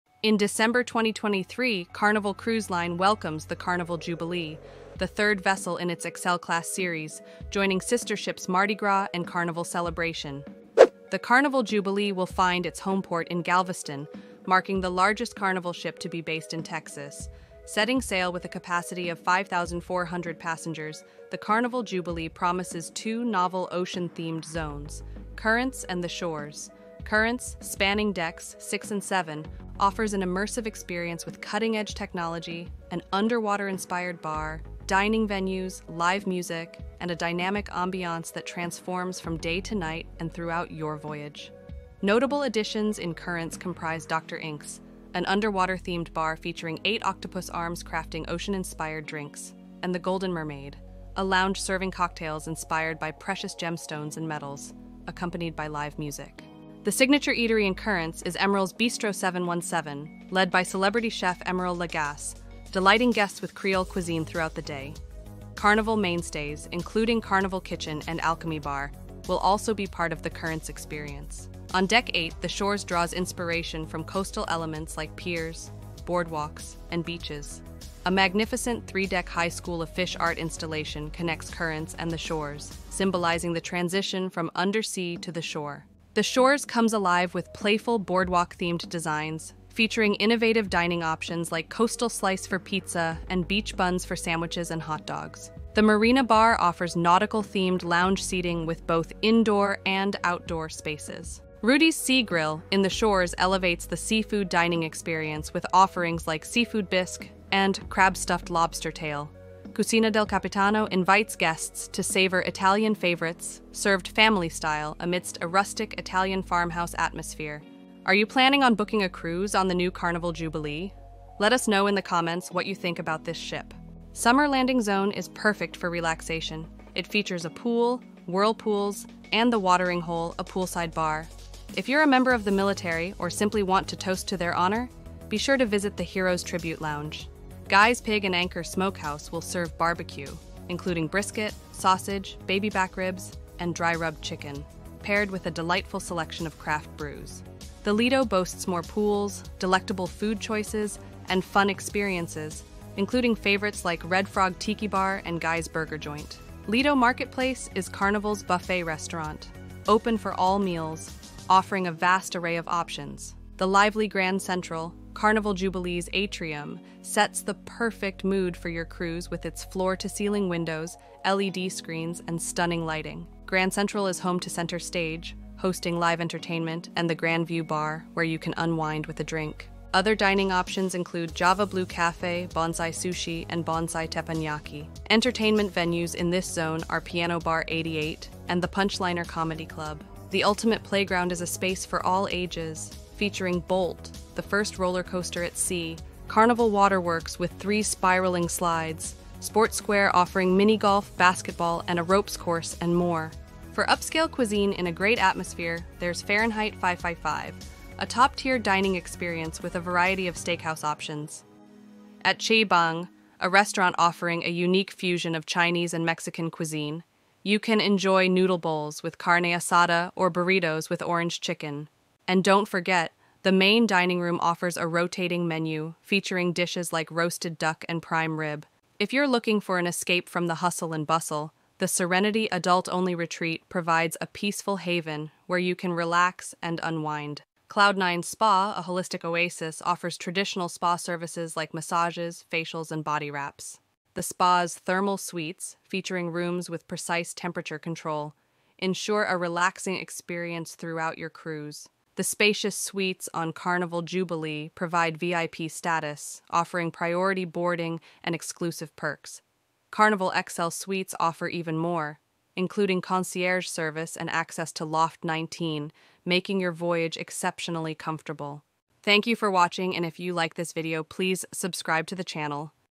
Carnival's new ship will call Galveston home. Carnival Jubilee Preview.
In December 2023, Carnival Cruise Line welcomed the Carnival Jubilee, the third vessel in its Excel-class series, joining sister ships Mardi Gras and Carnival Celebration. The Carnival Jubilee has its homeport in Galveston, marking the largest Carnival ship to be based in Texas.

If you enjoy the video, please make sure to give it a BIG THUMBS UP!

Take a look at the new Carnival Jubilee setting sail this December from Galveston, Texas. Do you like resort style ships? Let us know!

Setting sail with a capacity of 5,400 passengers, the Carnival Jubilee promises two novel ocean-themed zones: Currents and The Shores.
Currents, spanning decks 6 and 7, offers an immersive experience with cutting-edge technology, an underwater-inspired bar, dining venues, live music, and a dynamic ambiance that transforms from day to night and throughout your voyage. Notable additions in Currents comprise Dr. Inks, an underwater-themed bar featuring eight octopus arms crafting ocean-inspired drinks, and the Golden Mermaid, a lounge serving cocktails inspired by precious gemstones and metals, accompanied by live music.
The signature eatery in Currents is Emeril's Bistro 717, led by celebrity chef Emeril Lagasse, delighting guests with Creole cuisine throughout the day. Carnival mainstays, including Carnival Kitchen and Alchemy Bar, will also be part of the Currents experience.
On Deck 8, The Shores draws inspiration from coastal elements like piers, boardwalks, and beaches. A magnificent three-deck-high school of fish art installation connects Currents and The Shores, symbolizing the transition from undersea to the shore.
The Shores comes alive with playful boardwalk-themed designs, featuring innovative dining options like Coastal Slice for pizza and Beach Buns for sandwiches and hot dogs. The Marina Bar offers nautical-themed lounge seating with both indoor and outdoor spaces. Rudi's Seagrill in The Shores elevates the seafood dining experience with offerings like seafood bisque and crab-stuffed lobster tail. Cucina del Capitano invites guests to savor Italian favorites served family-style amidst a rustic Italian-farmhouse atmosphere.
For relaxation and enjoyment, the ship's Summer Landing zone features a pool, whirlpools, The Watering Hole, and a poolside bar. GUY'S PIG and ANCHOR SMOKEHOUSE will serve barbeque, including brisket, sausage, baby back ribs, and dry-rubbed chicken, paired with a delightful selection of craft brews.
The Lido boasts more pools, delectable food choices, and fun experiences, including favorites like Red Frog Tiki Bar and Guy’s Burger Joint.
Lido Marketplace is Carnival's buffet restaurant, open for all meals, offering a vast array of options.

Princess cruise food
Icon of the Seas
Celebrity Beyond
Norwegian Viva
Sun Princess
Discovery Princess